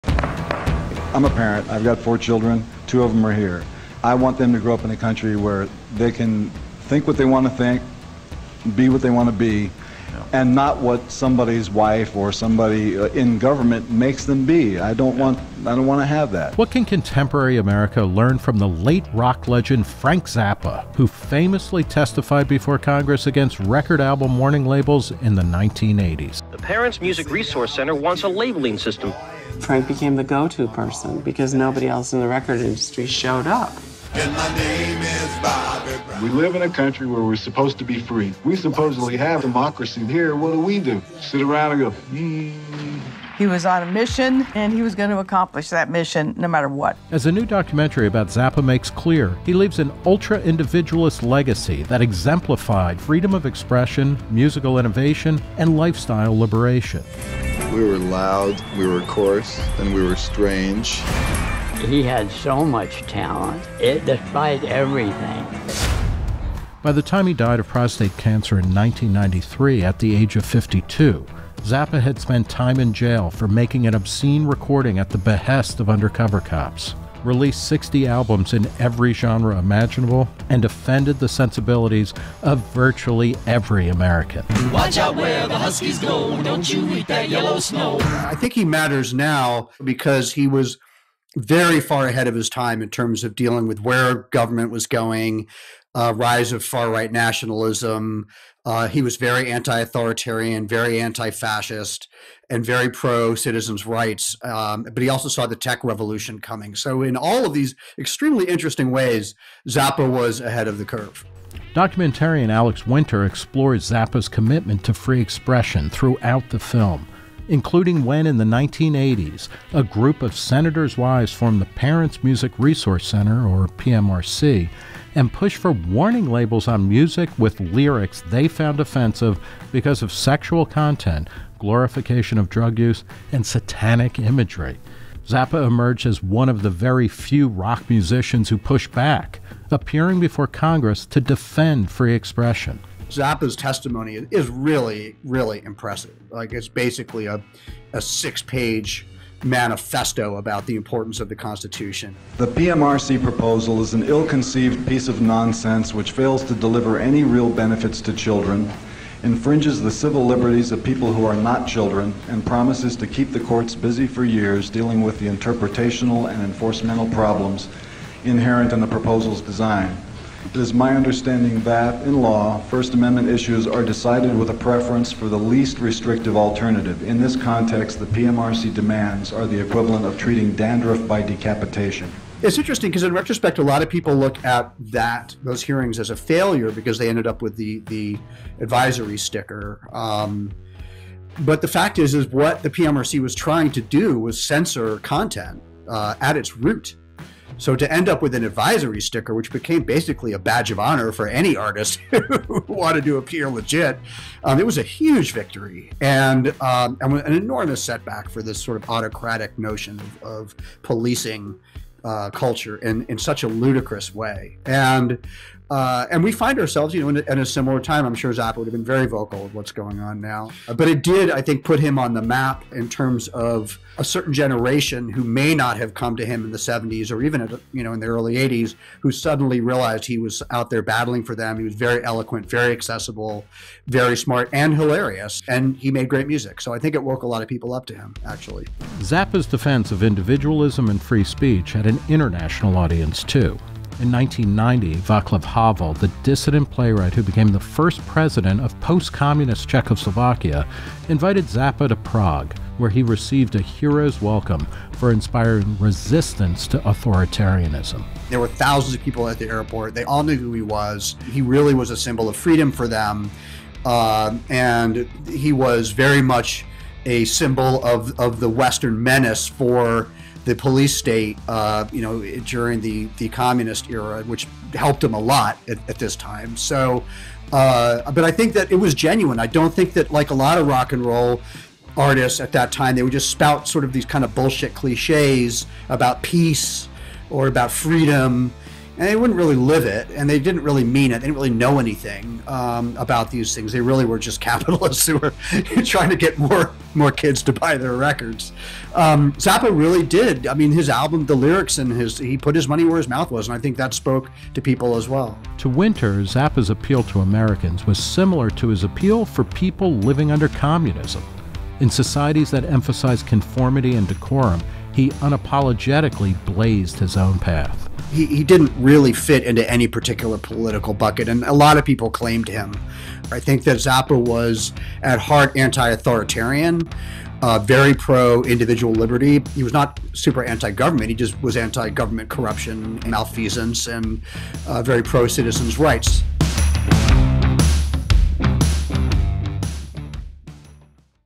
Rock Legend Frank Zappa Battles Censorship, Communism, and Conformity in a New Documentary
Alex Winter's new film celebrates the Rock Hall of Famer's individualism, anti-authoritarianism, and entrepreneurship.

Before his death from prostate cancer in 1993, Rock & Roll Hall of Fame member Frank Zappa spent time in jail for making an obscene recording requested by undercover cops, released 60 records in every genre imaginable, became one of the first modern musical artists to start his own company, testified before Congress that labeling music due to lyrical content was an attack on free speech, and inspired Vaclav Havel and others fighting Czechoslovakia's repressive communist regime. He packed a hell of a lot into 52 years, and made a lot of waves and enemies along the way.

Zappa is the subject of an eponymous new documentary by Alex Winter, whose previous films include Downloaded (a study of Napster and unauthorized file sharing), Deep Web (a look at Ross Ulbricht and the Silk Road), and Trust Machine (an exploration of how blockchain technology decentralizes power; go here for a Reason interview with him about that film).

"Zappa," says Winter, who is also well-known for his acting in the Bill & Ted series and other films, "matters because he was an extremely talented and composer but also because…he was very anti-authoritarian, very anti-fascist, very pro–citizens' rights. He also saw the tech revolution coming. In all of these extremely interesting ways, Zappa was ahead of the curve." Although his politics, like his music, defy easy (or any, really) categorization, Winter says that Zappa's overriding beliefs in free speech, individualism, and entrepreneurship carried strong libertarian connotations.

Narrated by Nick Gillespie; edited by Meredith Bragg.

Photo Credits: SONY PICTURES CLASSICS / Album/Newscom; Album / Francesc Fàbregas/Newscom; Philippe Gras/ZUMA Press/Newscom; Keystone Press Agency/ZUMA Press/Newscom; SONY PICTURES CLASSICS / Album/Newscom; Philippe Gras / Le Pictorium/Newscom; Rowntree, Bill/Mirrorpix/Newscom; KEYSTONE Pictures USA/ZUMAPRESS/Newscom; Alain Dister/DAPR/ZUMAPRESS/Newscom; Tschiponnique Skupin/ZUMApress/Newscom; Pamela Price/ZUMA Press/Newscom; Mirrorpix/Newscom